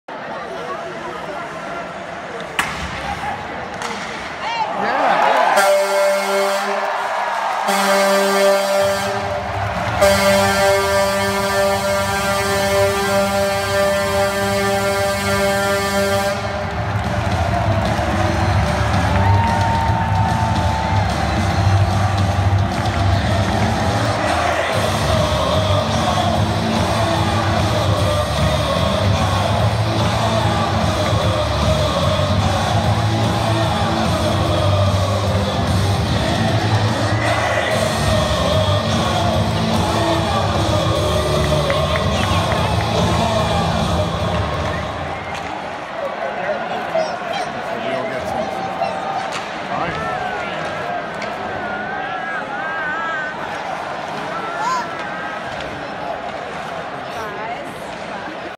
Phantoms vs. Senators 11/25/16 Goal 2
Mark Zengerle (5) ASST: Cole Bardreau (2), Greg Carey (4)